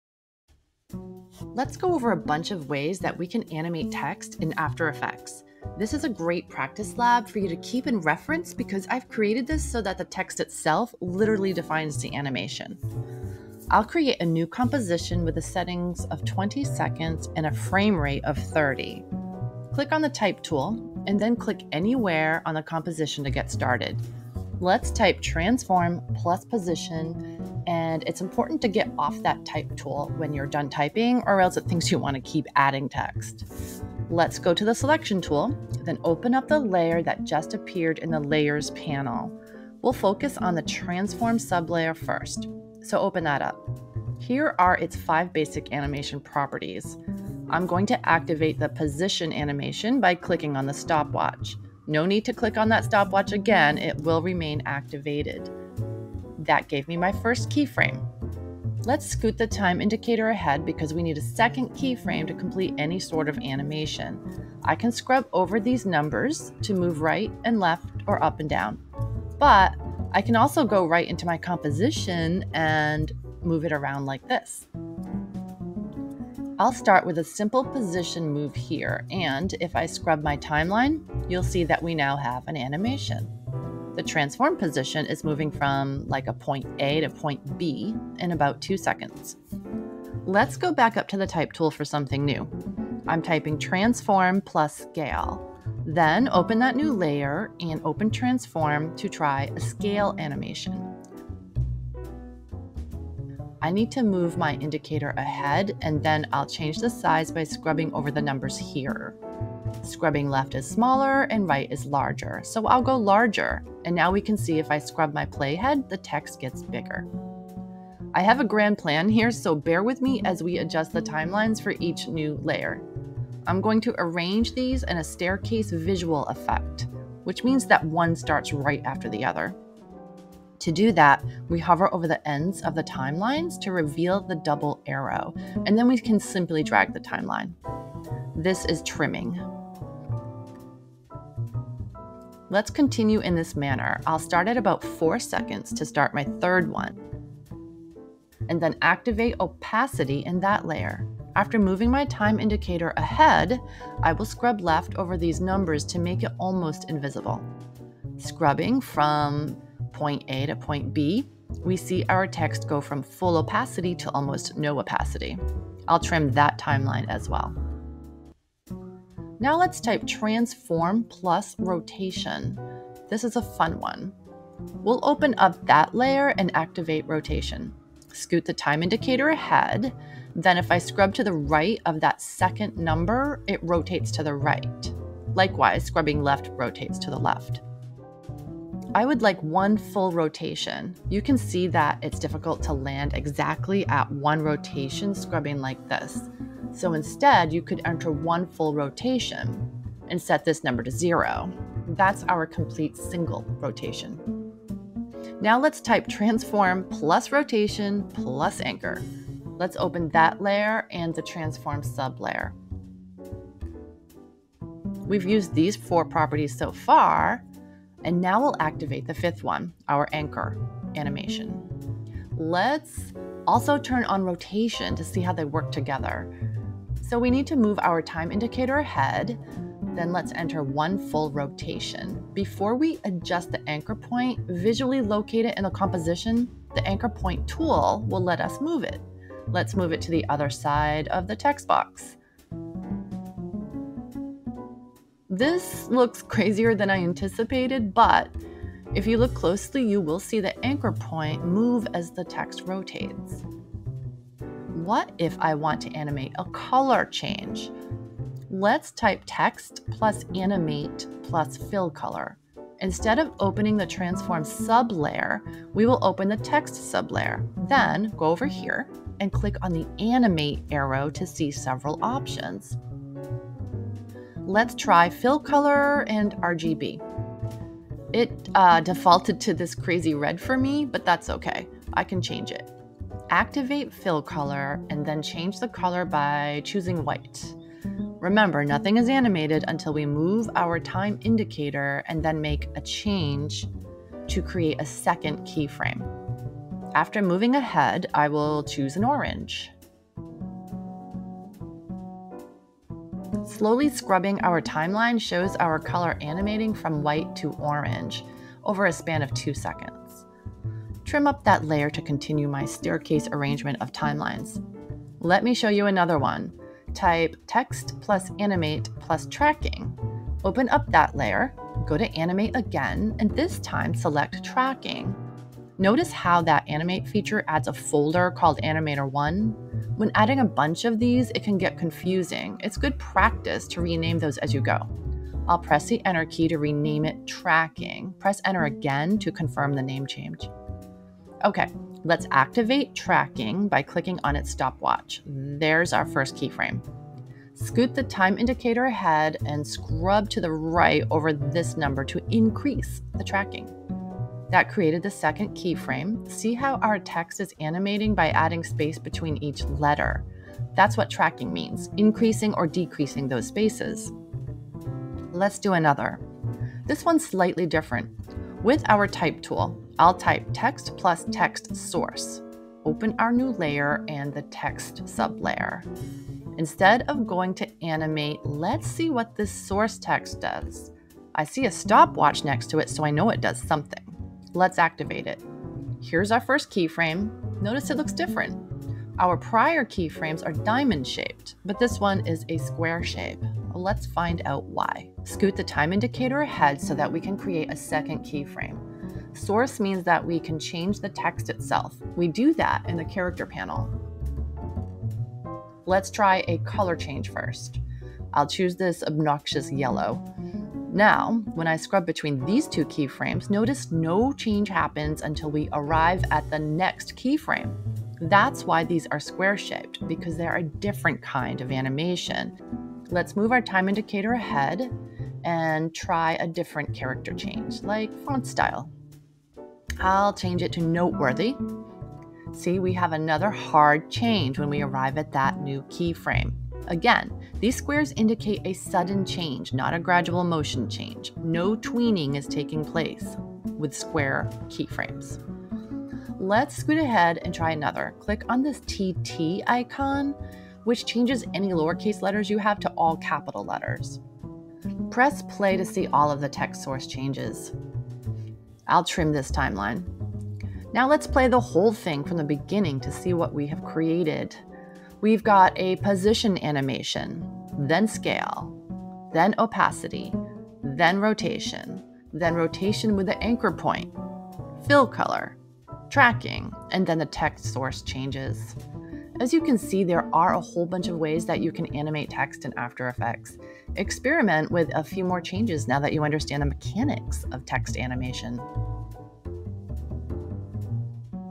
How to Animate Text in AFTER EFFECTS // Beginner Type Animation Tutorial
We'll go over a bunch of ways that we can animate text in After Effects. This is a great practice lab for you to keep and reference because I’ve created this so that the text itself literally references the chosen animation.